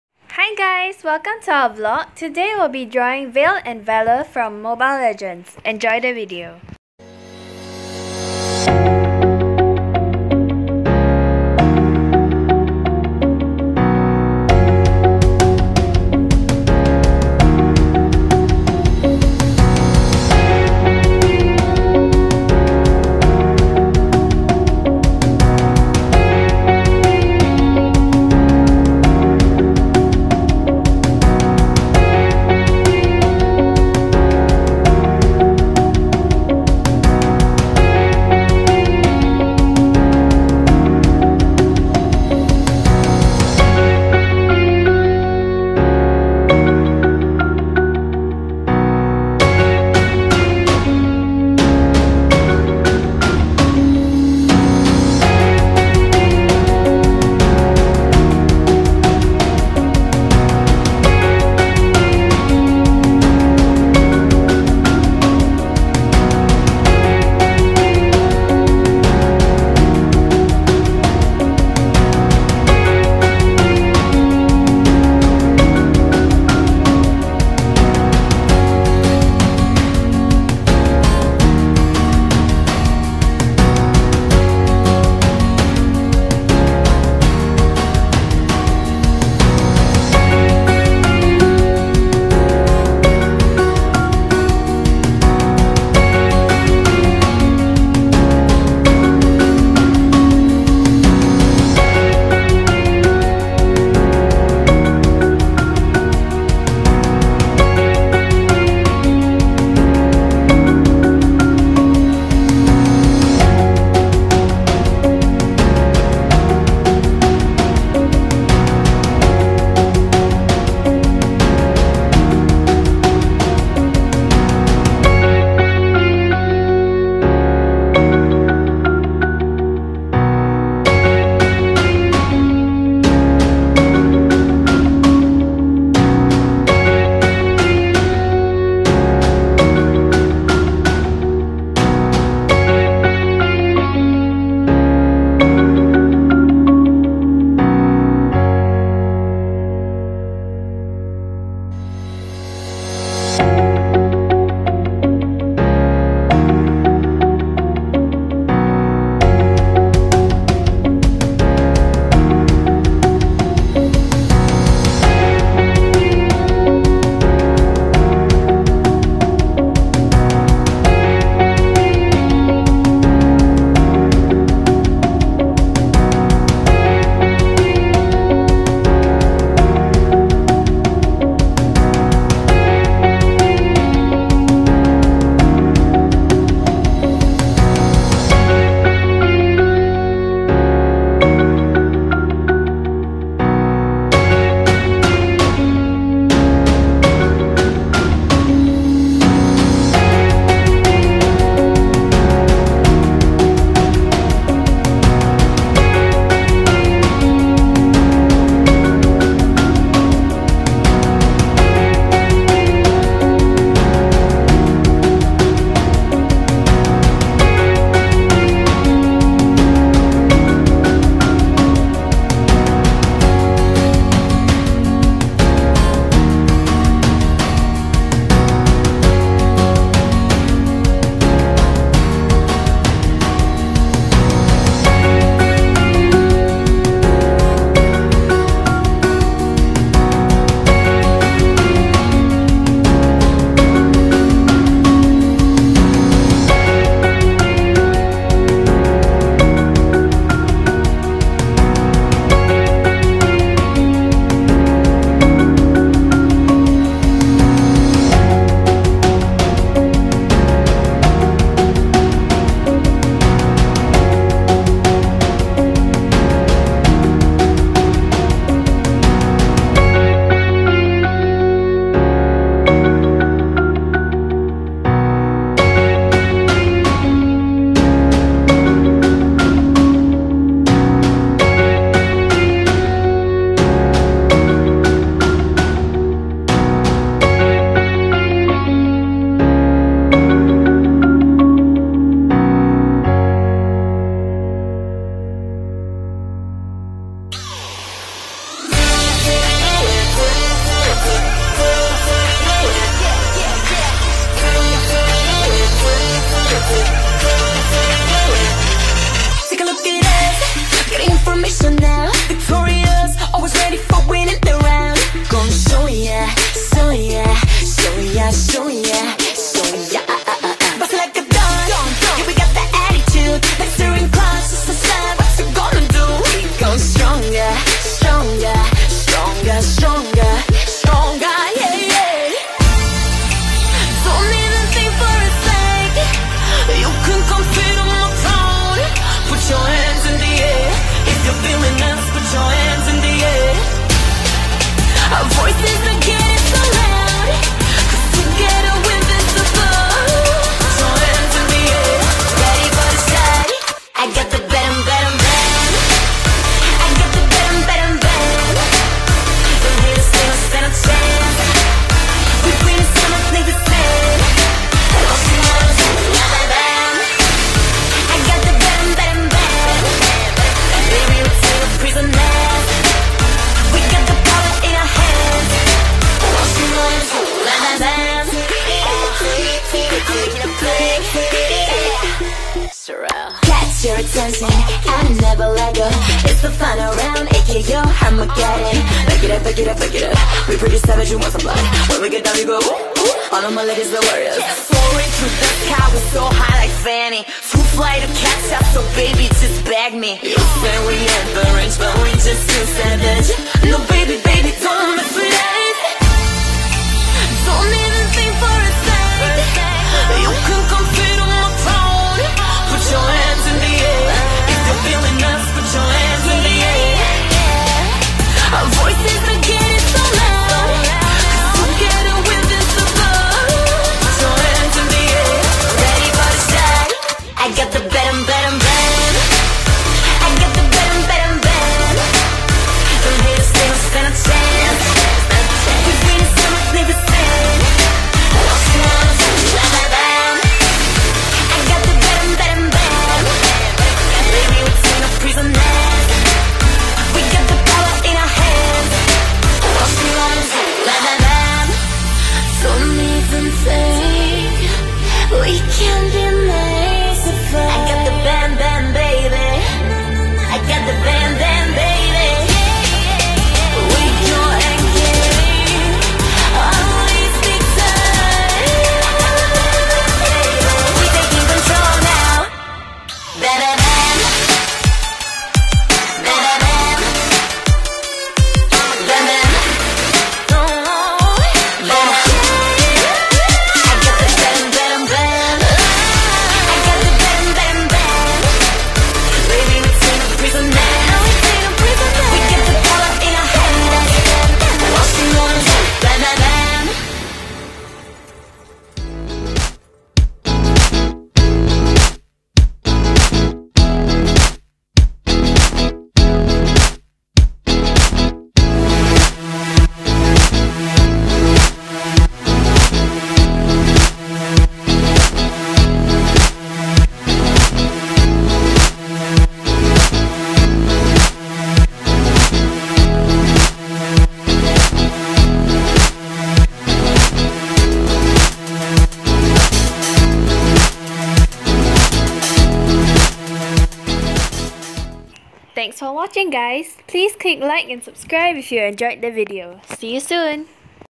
Drawing Vale and Valir | Mobile Legends
VLOG 60
Hi guys, today we will be drawing Vale and Valir from Mobile legends ,enjoy the video!!